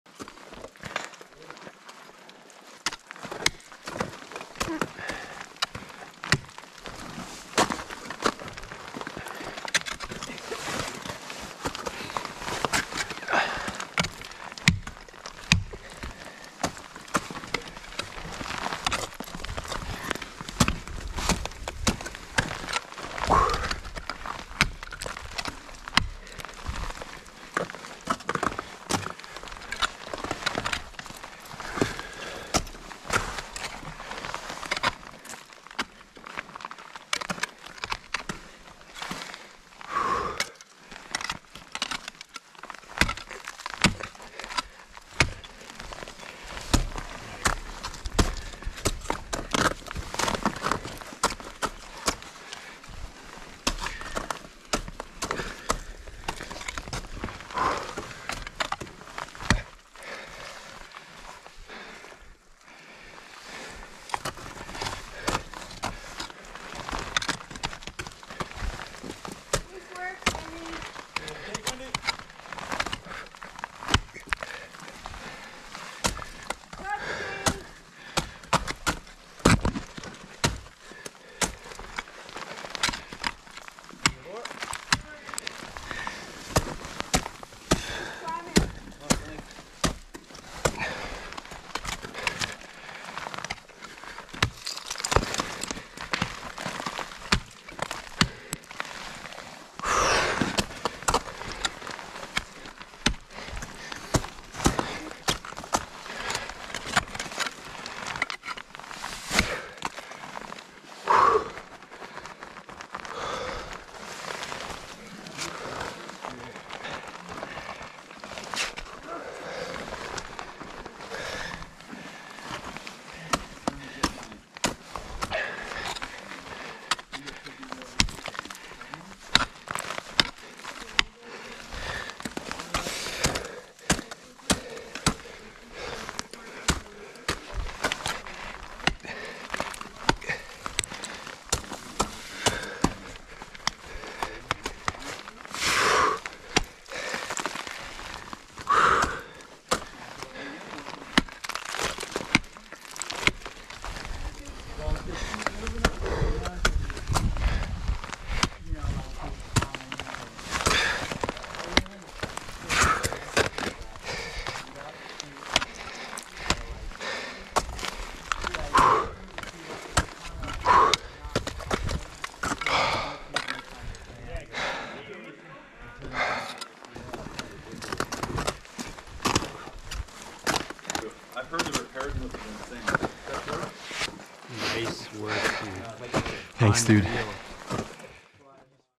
What It's Like Ice Climbing a Frozen Waterfall (POV)
First-person POV Ice Climbing at Bear Spirit Falls, near Banff, Alberta, Canada.

This route is on a very enjoyable, steep, relatively thin curtain at Bear Spirit Falls. The area is known for beautiful frozen waterfalls with some of the best top rope-friendly ice and mixed routes near Banff. As I continue to learn how to ice climb as a beginner, these areas are fantastic to work on my ice climbing technique.